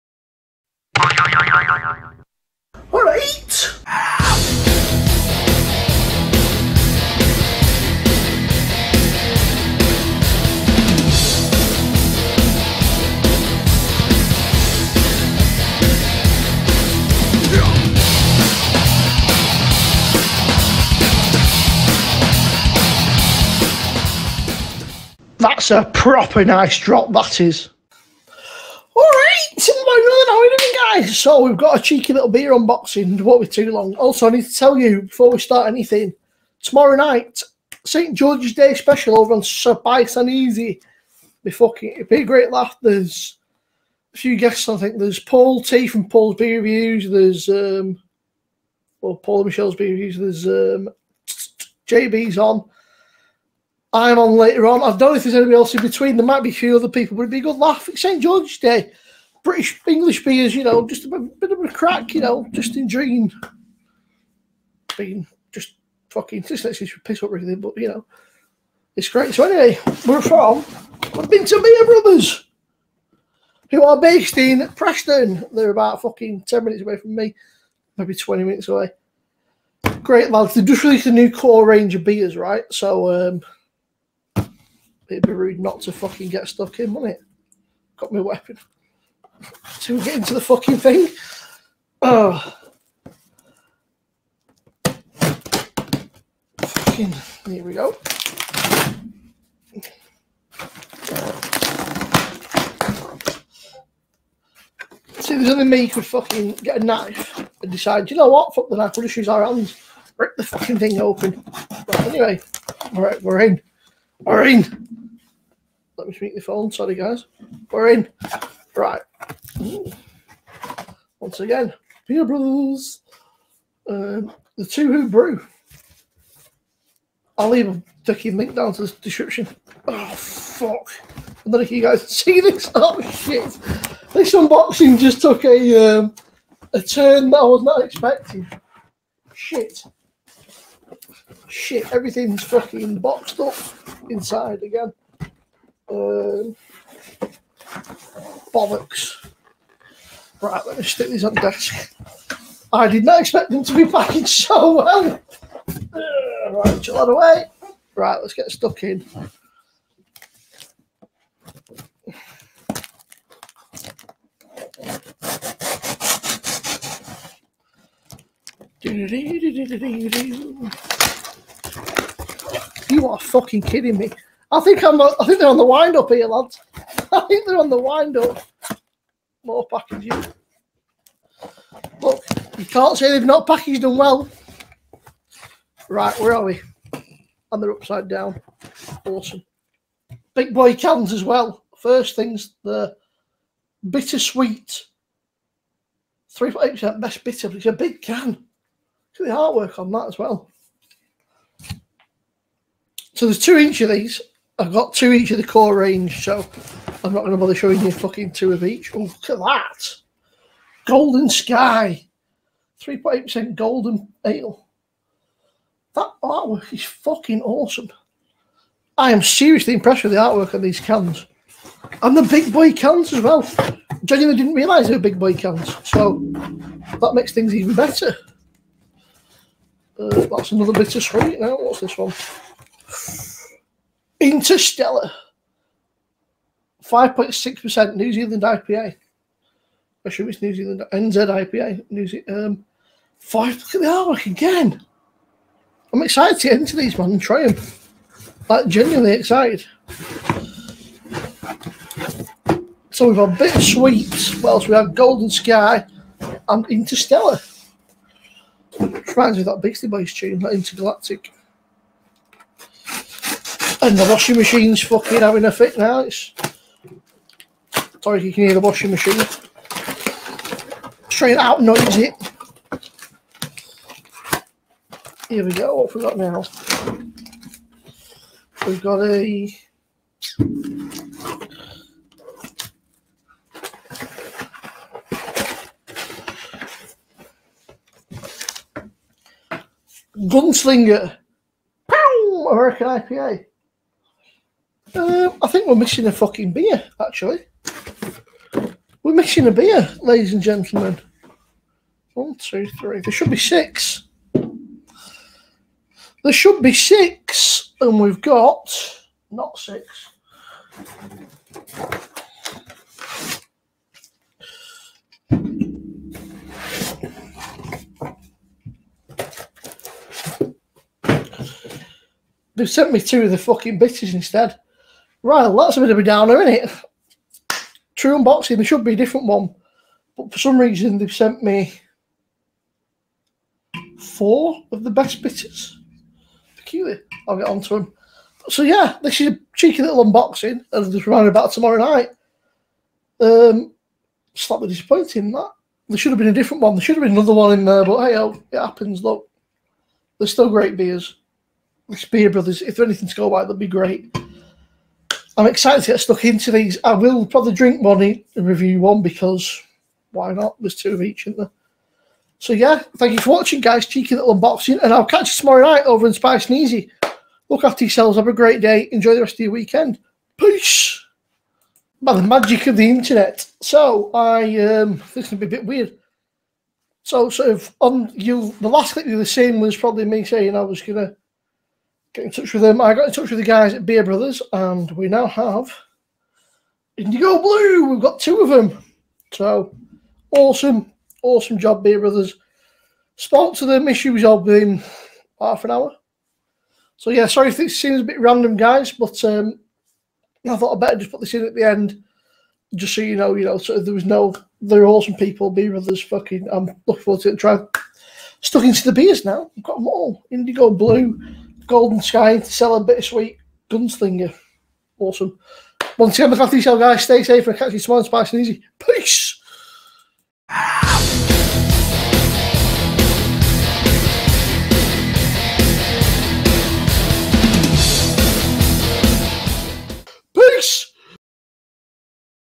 Beer Brothers NEW Core Range UNBOXING
Join me as we do a quick unboxing of the brand NEW Core Range from Beer Brothers in Preston.